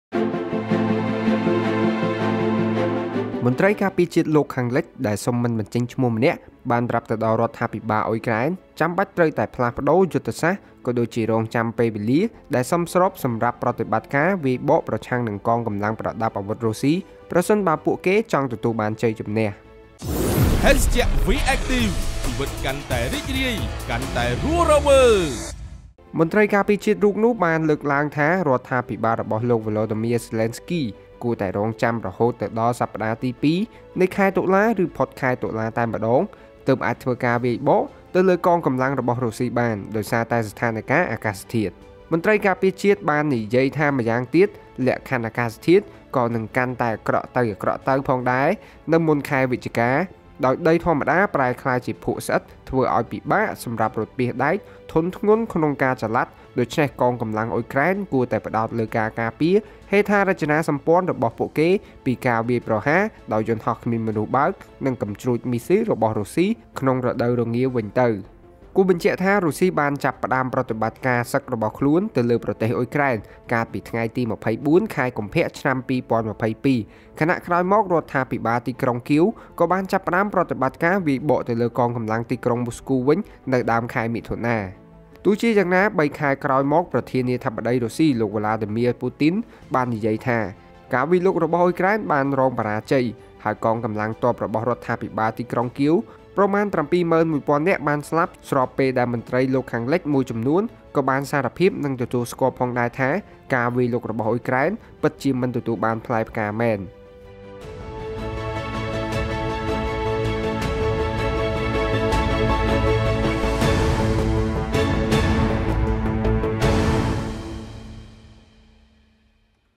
លោកខាងលិច ប្រាប់ អ៊ុយក្រែន ឲ្យប្ដូរយុទ្ធសាស្ត្រវាយបកលើ រុស្ស៊ី បើចង់ឈ្នះ
រង់ចាំពេលវេលាដែលសមស្របសម្រាប់ប្រតិបត្តិការវាយបកប្រឆាំងនឹងកងកម្លាំងប្រដាប់អាវុធរុស្ស៊ី ប្រសិនបើពួកគេចង់ទទួលបានជ័យជម្នះ ។